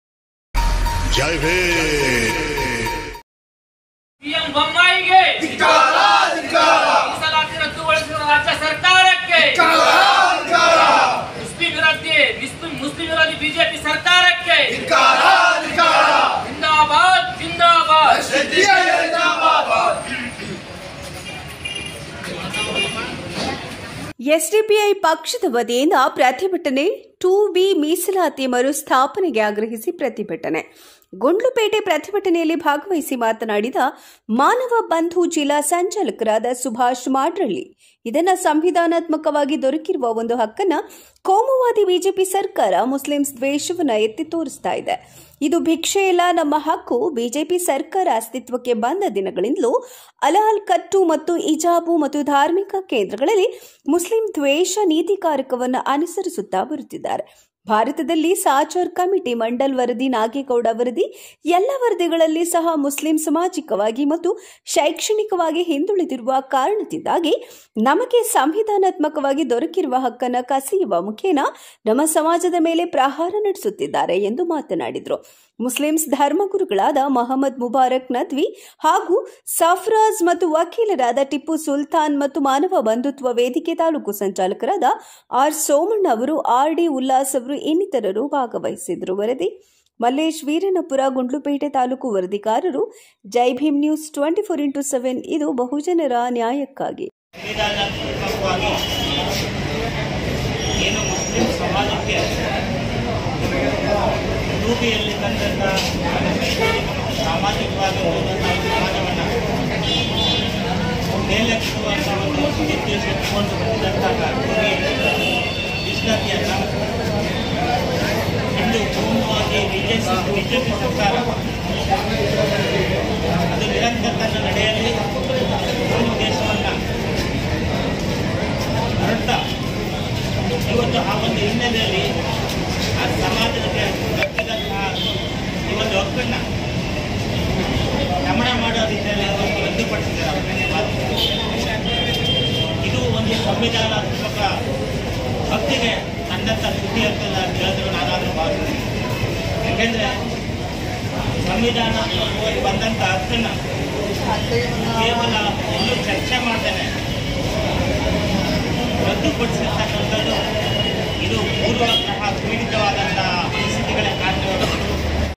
SDPI ಪಕ್ಷದ ವತಿಯಿಂದ ಪ್ರತಿಭಟನೆ 2ಬಿ ಮೀಸಲಾತಿ ಮರು ಸ್ಥಾಪನೆಗೆ ಆಗ್ರಹಿಸಿ ಪ್ರತಿಭಟನೆ
ಚಾಮರಾಜನಗರ ಗುಂಡ್ಲುಪೇಟೆ
ಹೆಡ್ಲೈನ್ಸ್
*ಎಸ್ ಡಿ ಪಿ ಐ ಪಕ್ಷದ ವತಿಯಿಂದ ಪ್ರತಿಭಟನೆ 2ಬಿ ಮೀಸಲಾತಿ ಮರು ಸ್ಥಾಪನೆಗೆ ಆಗ್ರಹಿಸಿ ಪ್ರತಿಭಟನೆ*

ಗುಂಡ್ಲುಪೇಟೆ ಪ್ರತಿಭಟನೆಯಲ್ಲಿ ಭಾಗವಹಿಸಿ ಮಾತನಾಡಿದ ಮಾನವ ಬಂದು ಜಿಲ್ಲಾ ಸಂಚಾಲಕರಾದ ಸುಭಾಷ್ ಮಾಡ್ರಳ್ಳಿ ಅವರು ಇದನ್ನು ಸಂವಿಧಾನಾತ್ಮಕವಾಗಿ ದೊರಕಿರುವಂತ ಈ ಒಂದು ಹಕ್ಕನ್ನು ಕೋಮುವಾದಿ ಬಿಜೆಪಿ ಸರ್ಕಾರ ಮುಸ್ಲಿಮ್ಸ್ ದ್ವೇಷವನ್ನು ಎತ್ತಿ ತೋರಿಸುತ್ತದೆ ಒಂದು ಭಿಕ್ಷೆಯಲ್ಲ ನಮ್ಮ ಹಕ್ಕು ಬಿಜೆಪಿ ಸರ್ಕಾರ ಅಸ್ತಿತ್ವಕ್ಕೆ ಬಂದ ದಿನಗಳಿಂದಲೂ ಅಲಾಲ್ ಕಟ್ಟು ಮತ್ತು ಇಜಾಬು ಮತ್ತು ಧಾರ್ಮಿಕ ಕೇಂದ್ರಗಳಲ್ಲಿ ಮುಸ್ಲಿಂ ದ್ವೇಷ ನೀತಿಕಾರಕವನ್ನು ಅನುಸರಿಸುತ್ತಾ ಬರುತ್ತಿದ್ದಾರೆ ಭಾರತದಲ್ಲಿ ಸಾಚಾರ್ ಕಮಿಟಿ ಮಂಡಲ್ ವರದಿ ನಾಗೇಗೌಡ ವರದಿ ಎಲ್ಲಾ ವರದಿಗಳಲ್ಲಿ ಸಹ ಮುಸ್ಲಿಮ್ ಸಾಮಾಜಿಕವಾಗಿ ಮತ್ತು ಶೈಕ್ಷಣಿಕವಾಗಿ ಹಿಂದುಳಿದಿರುವ ಕಾರಣದಿಂದಾಗಿ ನಮಗೆ  ಸಂವಿಧಾನಾತ್ಮಕ ದೊರಕಿರುವ ಹಕ್ಕನ್ನು ಕಸಿಯುವ ಮುಖಾಂತರ ನಮ್ಮ ಸಮಾಜದ ಮೇಲೆ ಪ್ರಹಾರ ನಡೆಸುತ್ತಿದ್ದಾರೆ ಎಂದು ಮಾತನಾಡಿದರು ಮುಸ್ಲಿಮ್ಸ್ ಧರ್ಮ ಗುರುಗಳಾದ ಮಹಮ್ಮದ್ ಮುಬಾರಕ್ ನದ್ವಿ ಹಾಗೂ ಸರ್ಫ್ರಾಜ್ ಮತ್ತು ವಕೀಲರಾದ ಟಿಪ್ಪು ಸುಲ್ತಾನ್ ಹಾಗೂ ಮಾನವ ಬಂಧುತ್ವ ವೇದಿಕೆ ತಾಲೂಕು  ಸಂಚಾಲಕರಾದ ಆರ್ ಸೋಮಣ್ಣನವರು ಆರ್ ಡಿ ಉಲ್ಲಾಸ್ ರವರು ಇನ್ನಿತರರು ಭಾಗವಹಿಸಿದರು

ವರದಿ:-ಮಲ್ಲೇಶ್ ವೀರನಪುರ ಗುಂಡ್ಲುಪೇಟೆ ತಾಲೂಕು ವರದಿಗಾರರು
ಜೈ ಭೀಮ್ ನ್ಯೂಸ್ 24X7 ಇದು ಬಹುಜನರ ನ್ಯಾಯಕ್ಕಾಗಿ